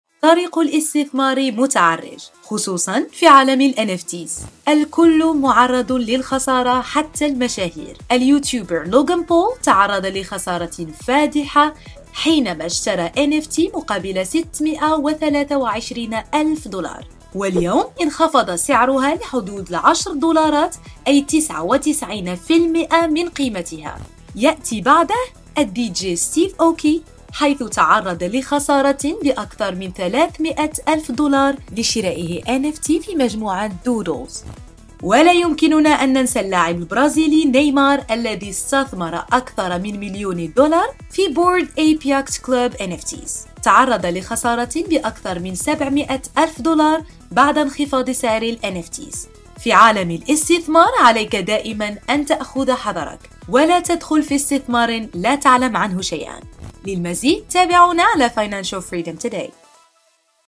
مشاهير في ال NFT
حتى المشاهير يخسرون في عالم الاستثمار من بينهم اللاعب البرازيلي نيمار، فما هو حجم خسارتهم يا ترى؟

طريق الاستثمار متعرج، خصوصًا في عالم الNFTs.
الكل معرض للخسارة حتى المشاهير،
اليوتيوبر لوغن بول تعرض لخسارة فادحة حينما اشترى NFT مقابل 623 ألف دولار.
واليوم انخفض سعرها لحدود ال10 دولارات أي 99% من قيمتها
يأتي بعده الدي جي ستيف أوكي، حيث تعرض لخسارة بأكثر من 300 ألف دولار لشرائه NFT في مجموعة Doodles.
ولا يمكننا أن ننسى اللاعب البرازيلي نيمار الذي استثمر أكثر من مليون دولار في Bored Ape Yacht Club NFTs
تعرض لخسارة بأكثر من 700 ألف دولار بعد انخفاض سعر الNFTs.
في عالم الاستثمار عليك دائماً أن تأخذ حذرك
ولا تدخل في استثمار لا تعلم عنه شيئًا.